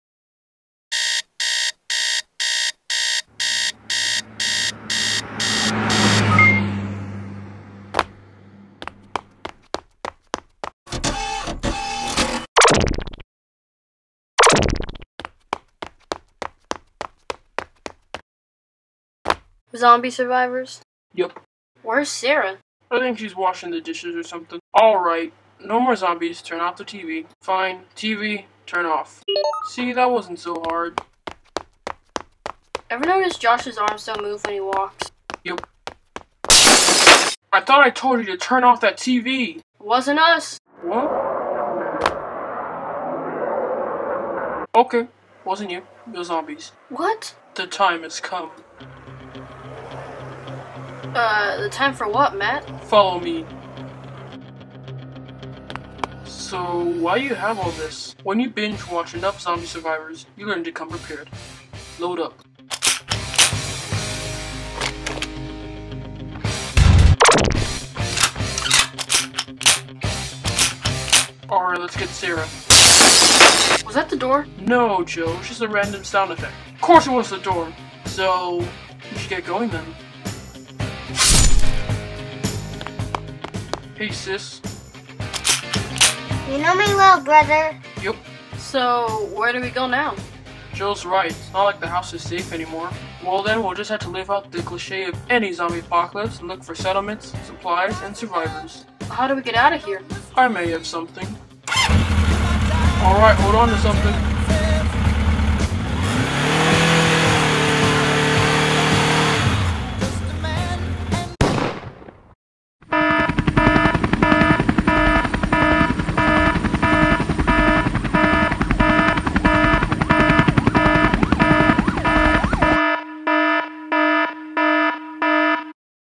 The Lego Zombie Apocalypse Ep 1: Pilot
Matt, Joe, Sara, and Josh are all caught in a Zombie invasion as a mysterious infection has begun.

Notice: All comments have been disabled for this video because people keep saying it’s a ripoff or copy when it’s not but merely an inspiration.